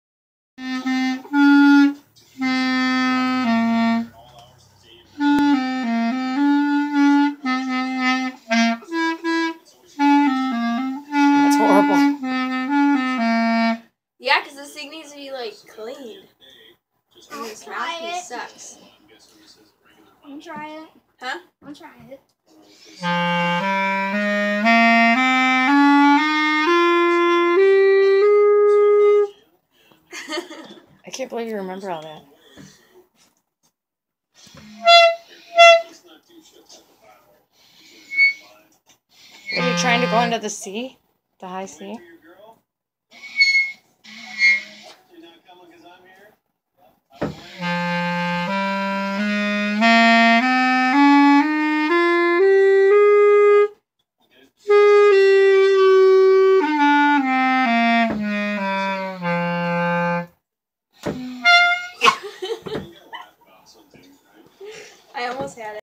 Caitlyn and the clarinet oct 2017
My little girl remembers how to play.. Just kidding she is awful.